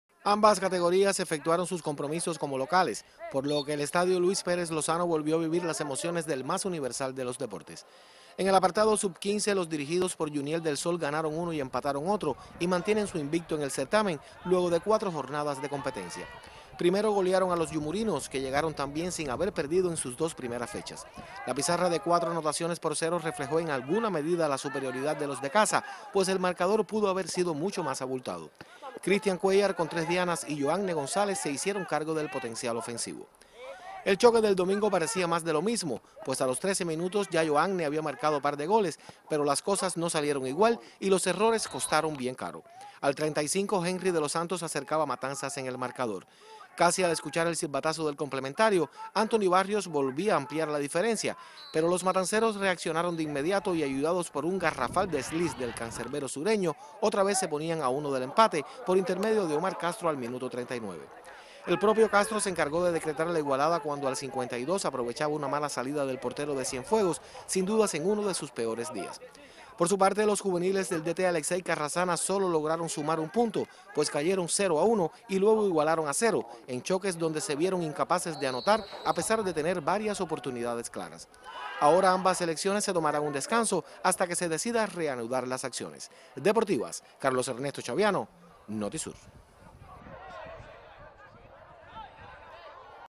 Cienfuegos sigue acaparando resultados en el fútbol escolar y juvenil
En lo que representó la última presentación del fútbol antes de la obligada parada debido a la compleja situación que atraviesa el país por la escasez de combustible, los equipos de Cienfuegos sacaron diferentes rentas en sus respectivos campeonatos nacionales.